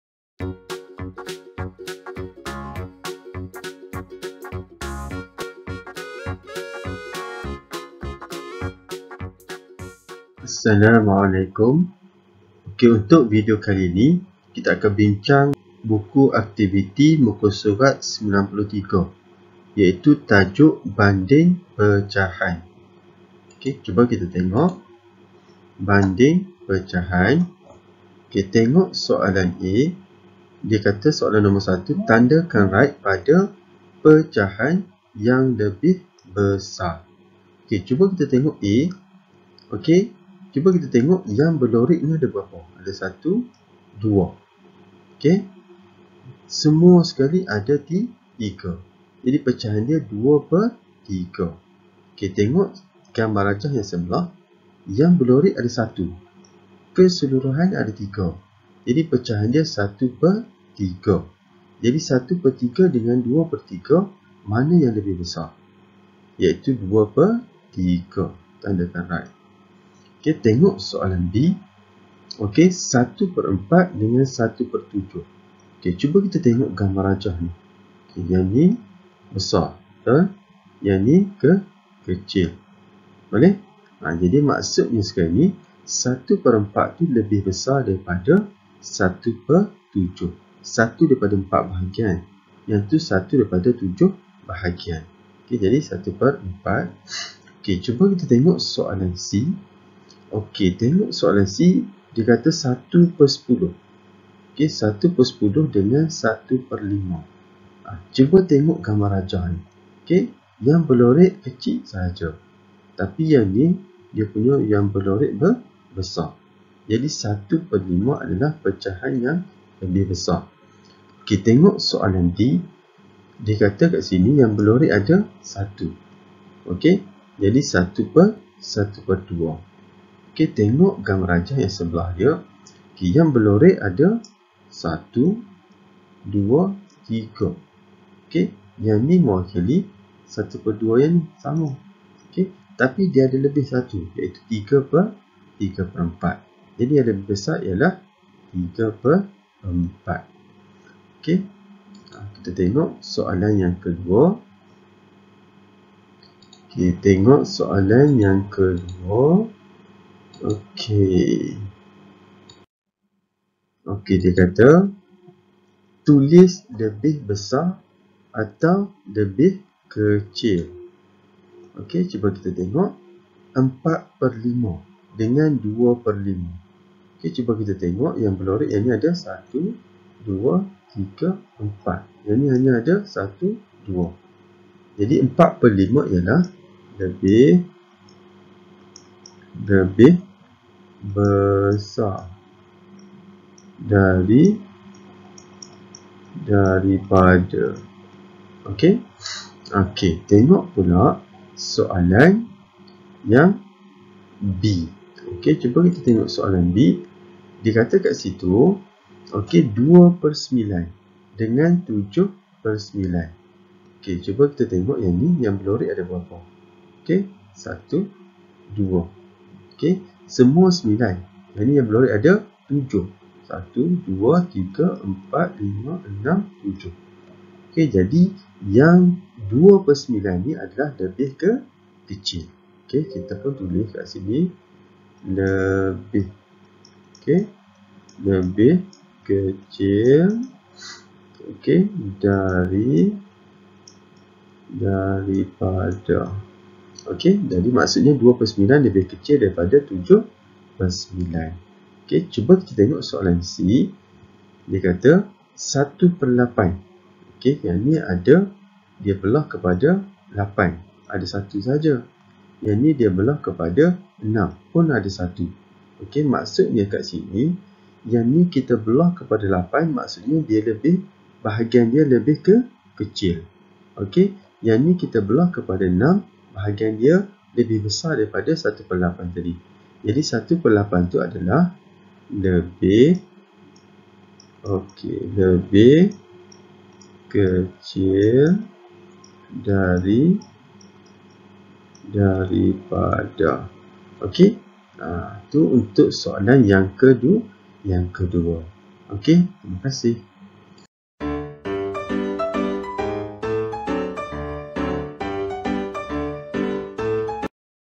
PERBINCANGAN BUKU AKTIVITI MUKA SURAT 93 BANDING PECAHAN MATEMATIK TAHUN 2
As salam & Salam sejahtera. Perbincangan Buku Aktiviti Matematik Tahun 2 Muka Surat 93. Tajuk Banding Pecahan. Terima kasih.